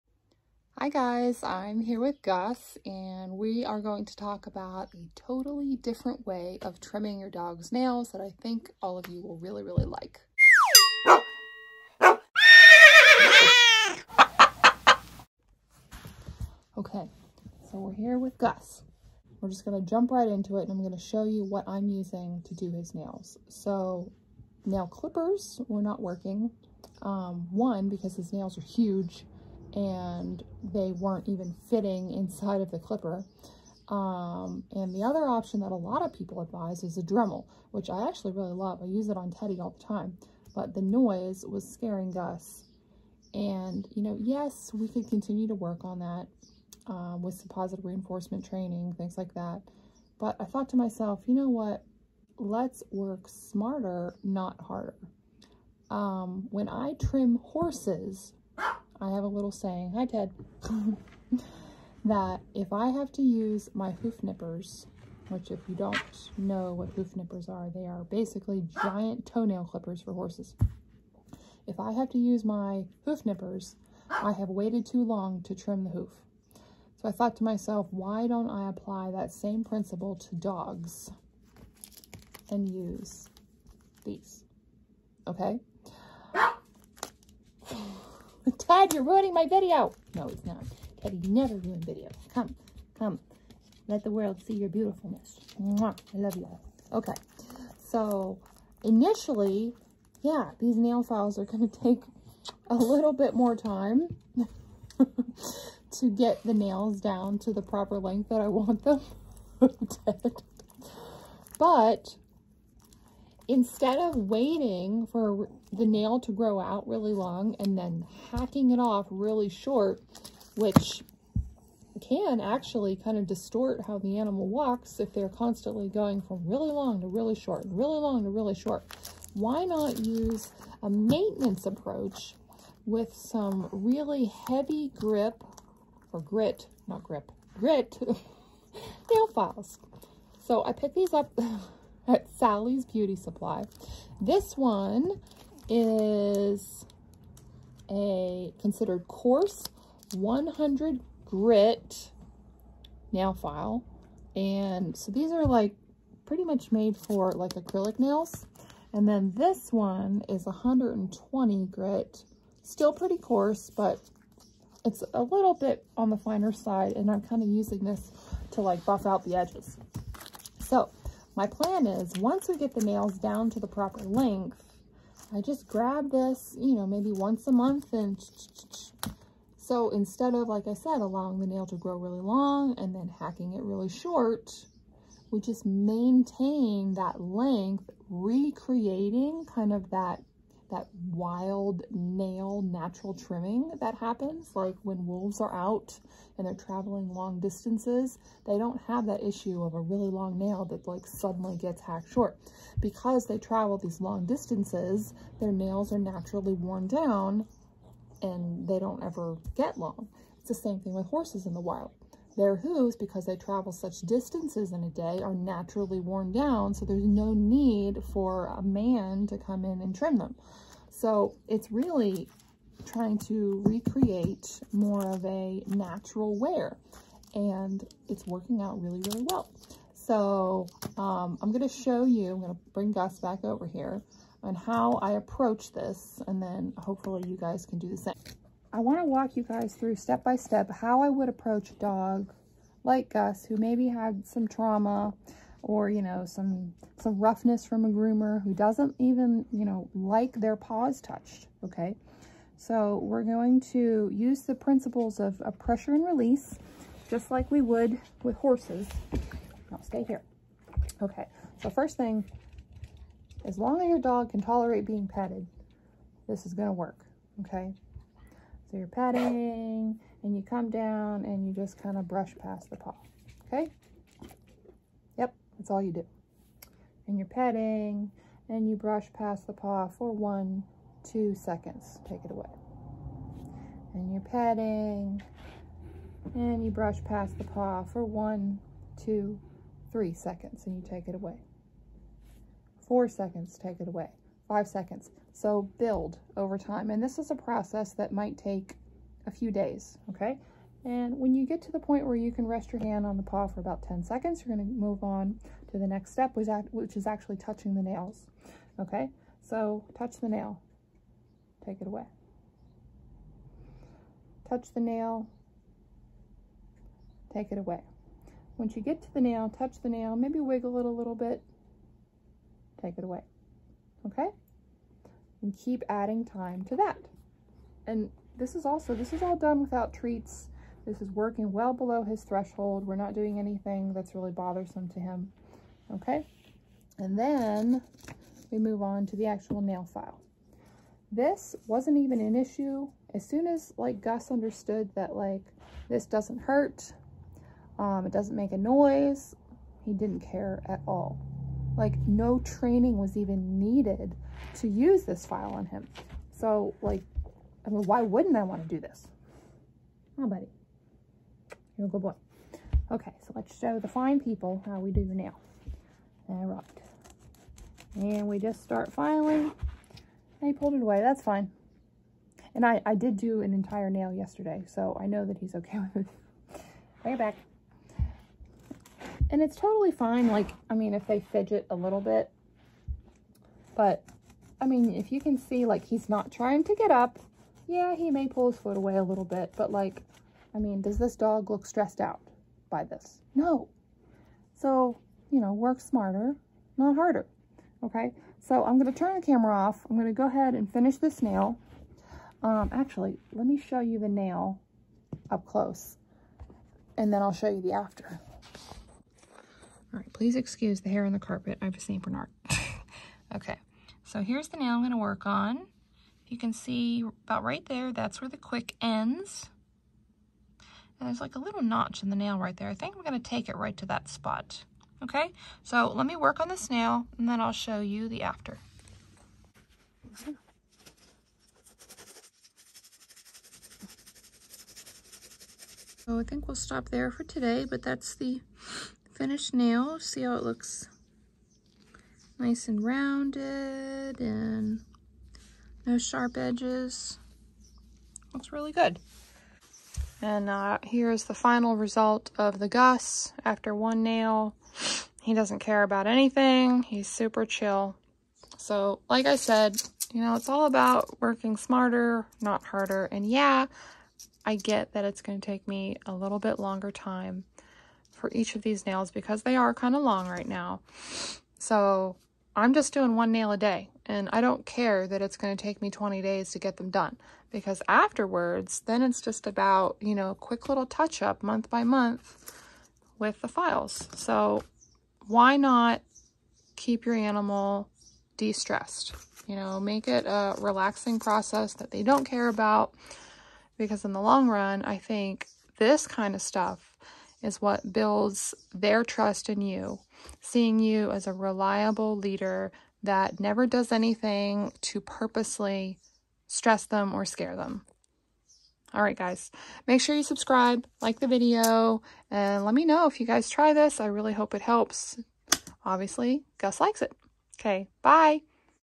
ZERO STRESS DOG NAIL TRIMMING
A completely new approach to trimming your dogs nails!